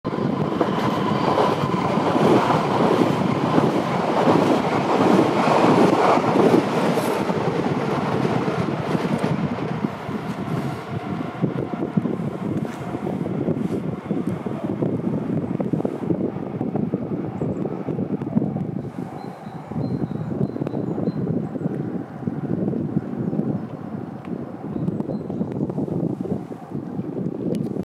SL pendeltåg at Södermalm Stockholm. (2023-01-14)
An Storstockholms Lokaltrafik pendeltåg/commuter train leaving the island of Södermalm.